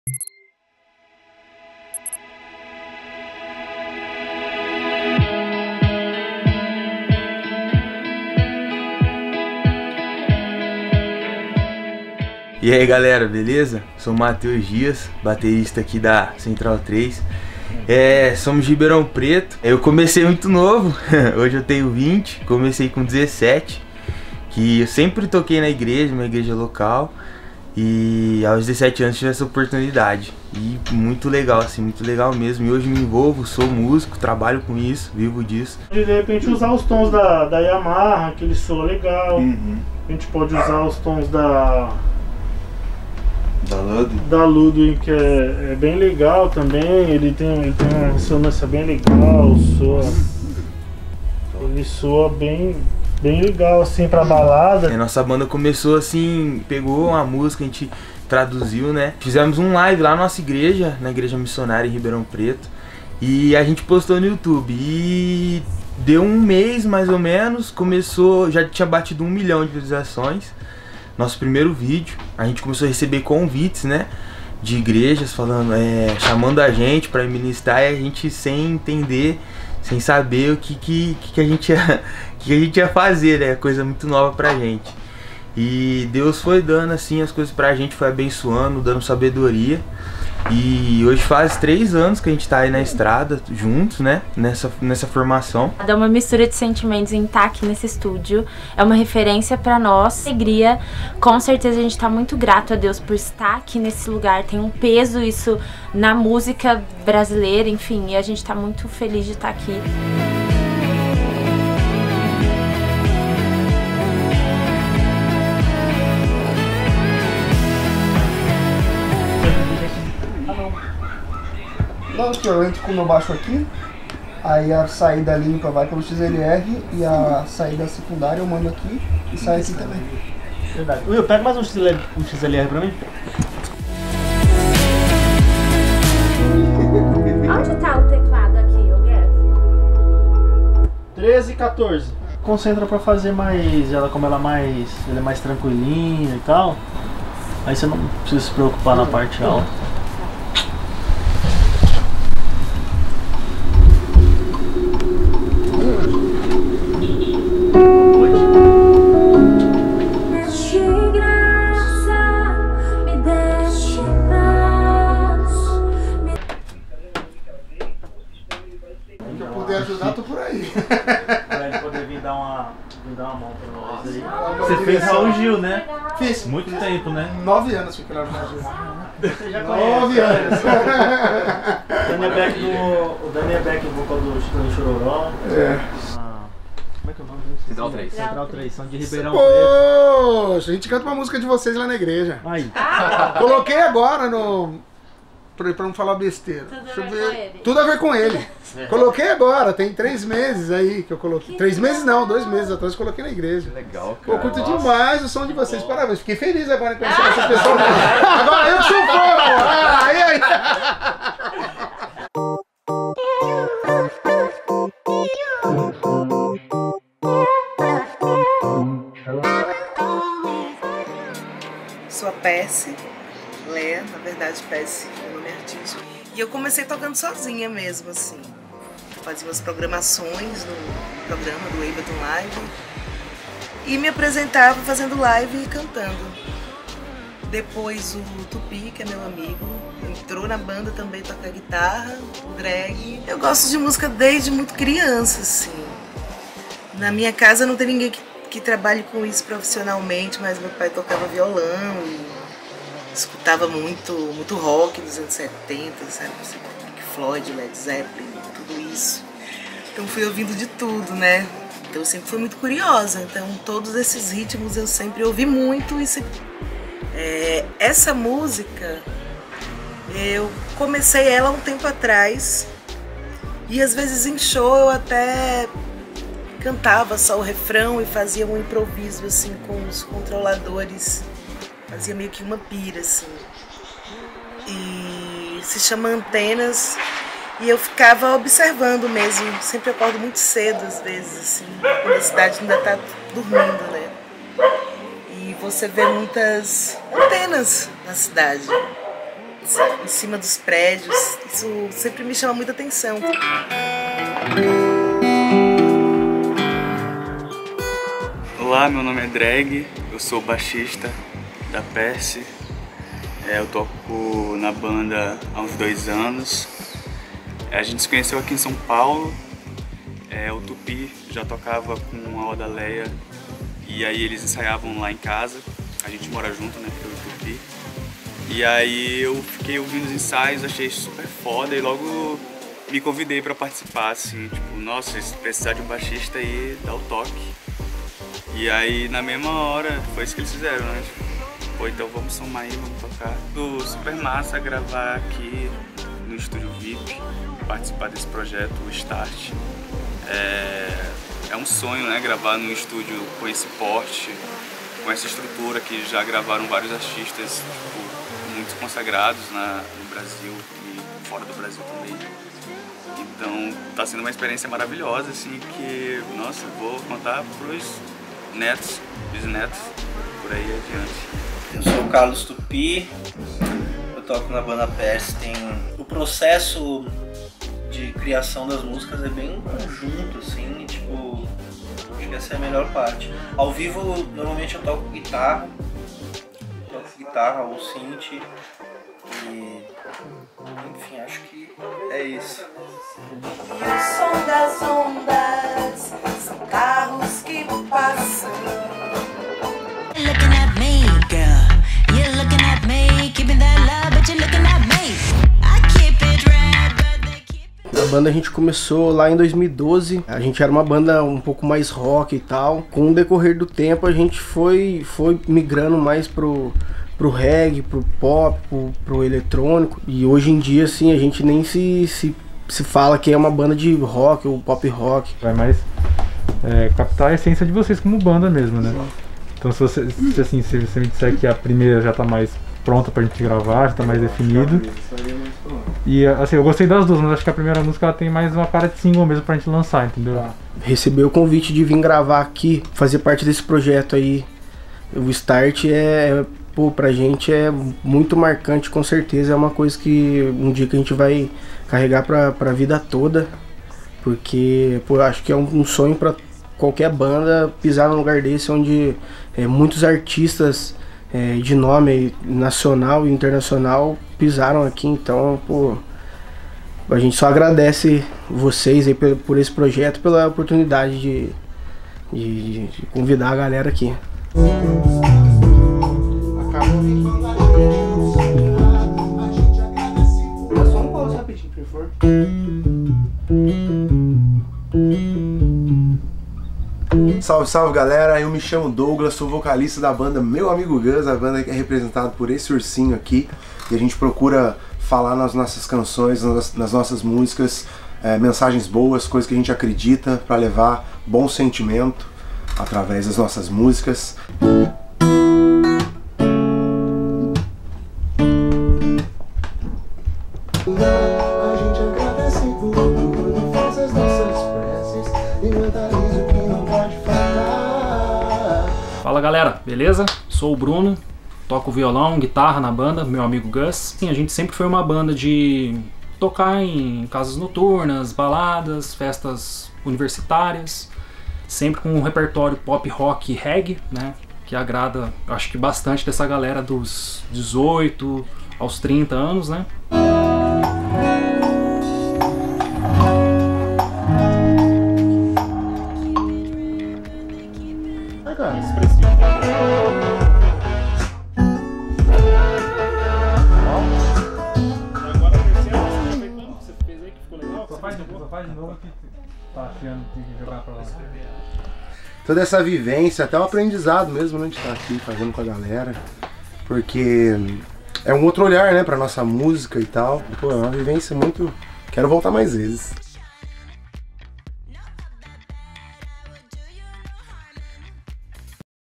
Backstage START - Central 3, Persie e Meu Amigo Gus
No Backstage Vip do START recortamos os melhores momentos da experiência de gravação cada banda que participou do projeto com a gente. Nesse mês tivemos, respectivamente, a presença da Dom Pescoço, Eixo Nacional, Laika Não Morreu! e Essência.
Assista e compartilhe as músicas, os vídeos e o backstage de dezembro!
Ficou curioso? Saiba mais sobre o START
Gravado e editado no Studio VIP - São Paulo/SP
Técnico de Gravação: Fernando Silvério "Mow" e Lucca Bruno
Equipe de Comunicação: Breno Wallace, Gabriele Oliveira e Júlia Campos